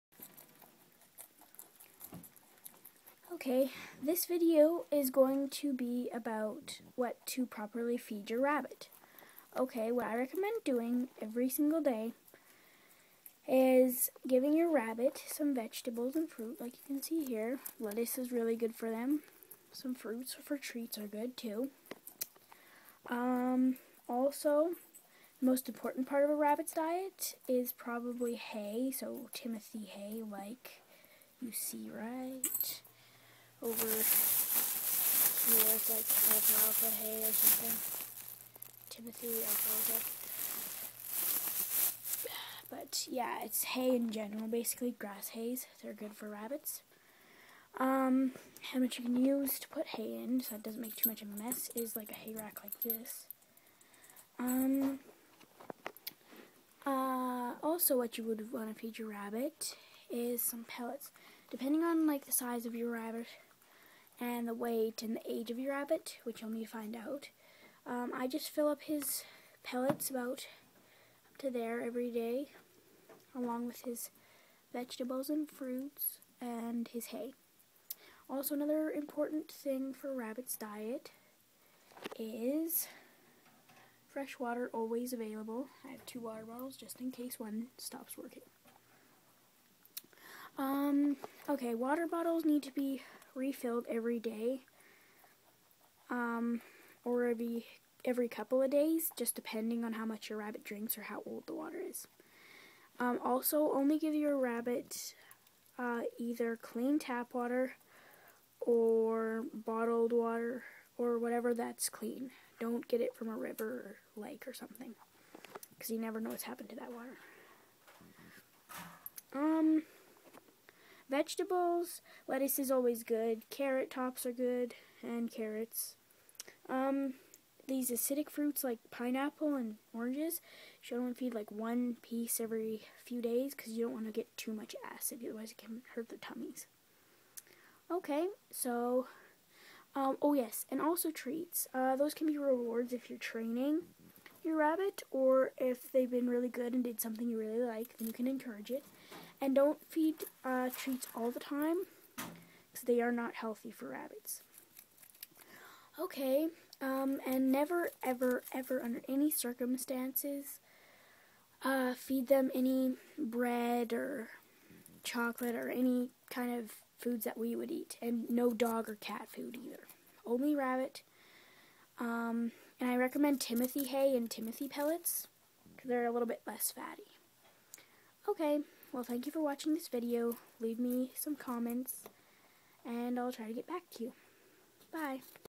The Proper Diet for a Rabbit
This video explains how and what to feed your pet rabbit.